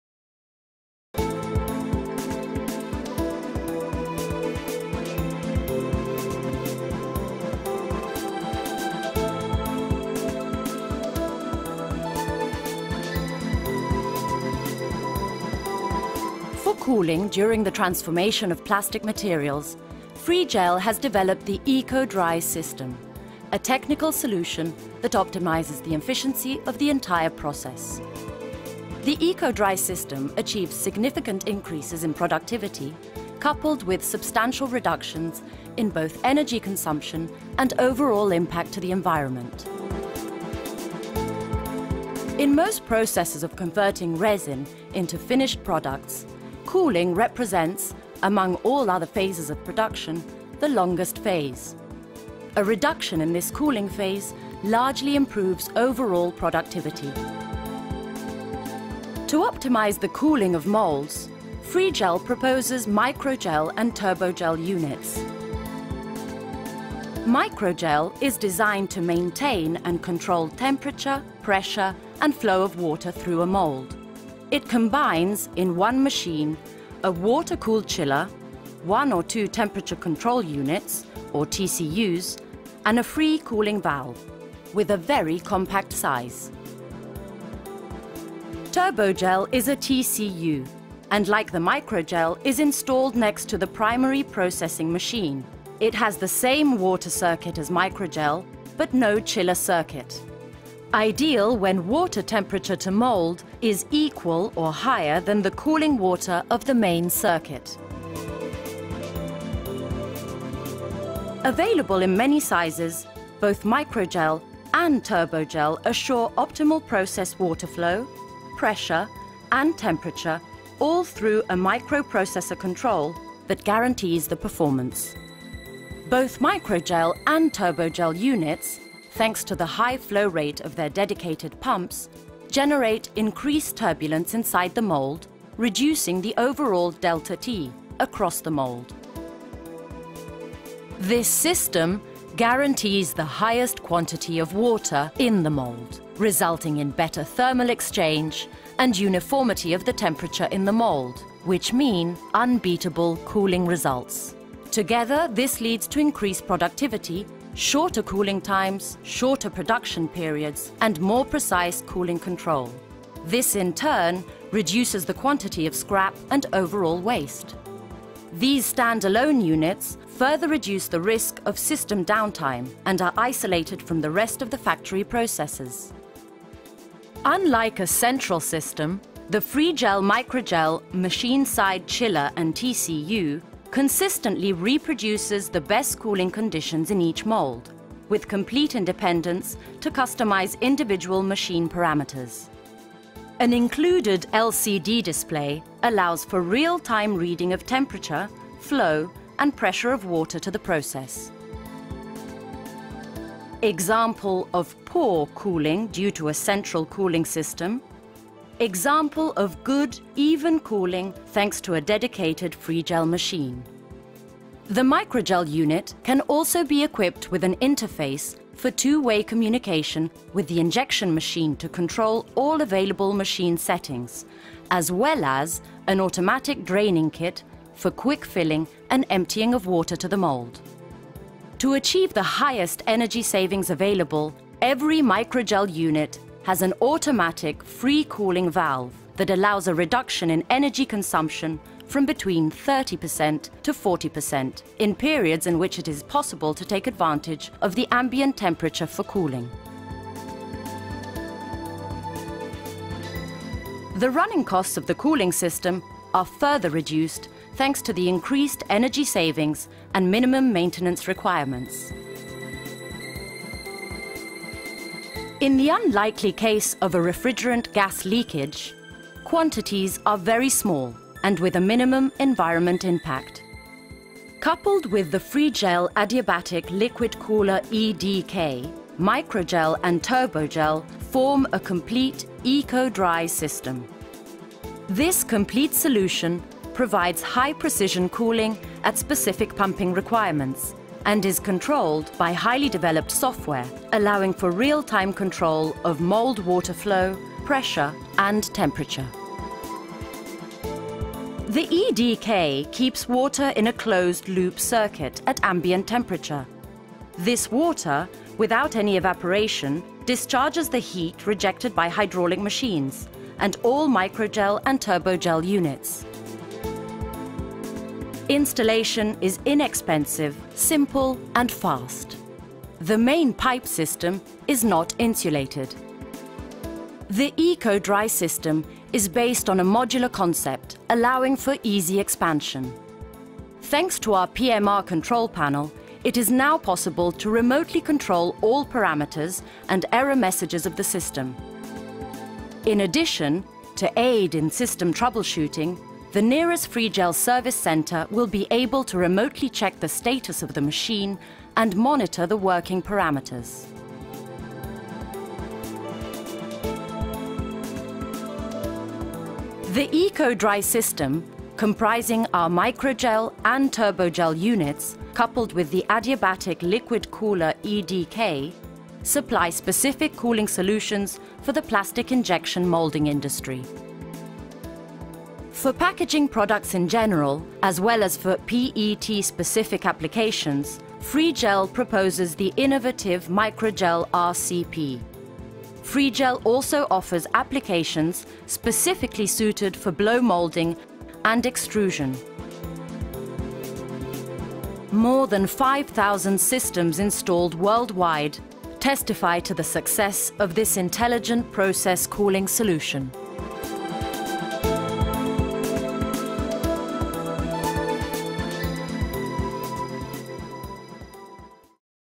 Intelligent Process Cooling for Plastics - with subtitles
This video explains the main features of Microgel process individual chillers and Turbogel temperature controllers.

Plastic Process Cooling Systems and Equipments

Our cooling system highly reduces cooling times with improved product quality in every application: injection, blow molding or extrusion.

Technologically advanced cooling system for injection molding of automobile components.